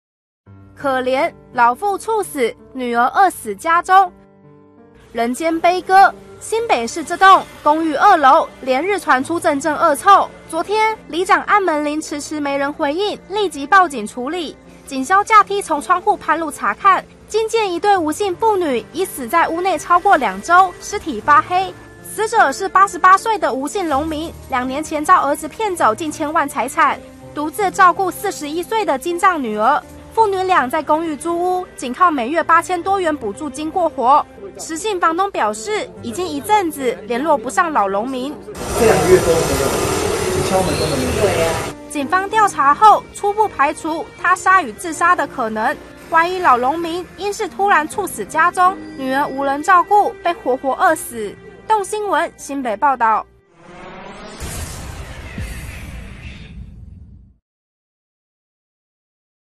可憐 父猝死 病女餓死家中 - 蘋果日報
【石永軒、黃楷棟╱新北報導】人間悲歌！新北市一名擁有近千萬元財產的八十八歲老榮民，兩年前被兒子拿走財產遠走中國後，他獨力照料四十一歲精障女兒，日前他疑猝死家中，女兒無人照顧竟活活餓死，鄰居昨聞到惡臭報案，警方入屋赫然發現父女倆已發黑、滲出屍水，冰箱空無一物，初判至少死亡逾兩周，鄰居不禁嘆：「真可憐！」